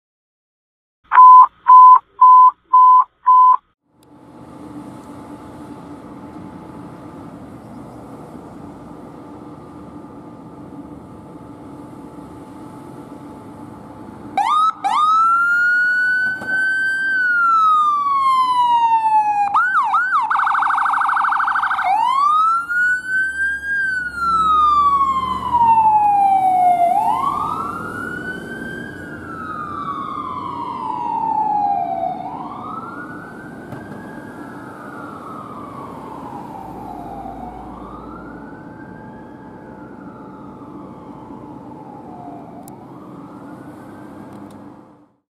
*PRIORITY TONE* Anaheim Fire & Rescue Ambulance 6 Responding
Ambulance 6 is seen here responding to a medical call with Truck 2.

Anaheim, CA - August 2022